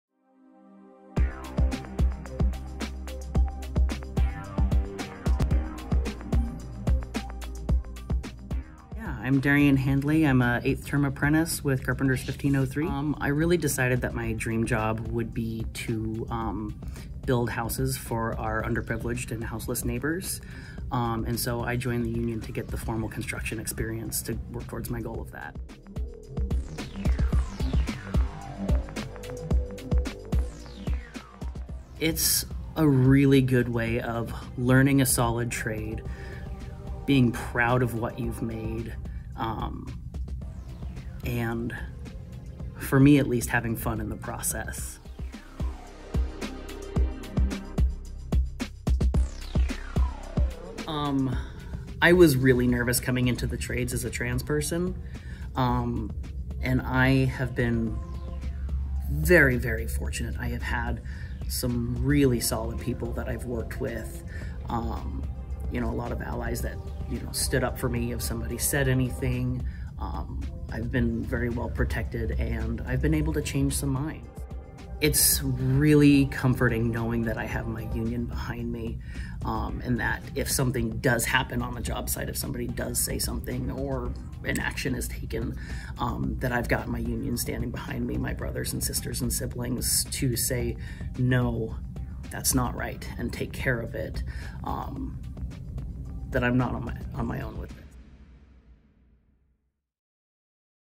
Meet Darien Handley, an apprentice with Carpenters 1503.
Keep working hard and never stop learning.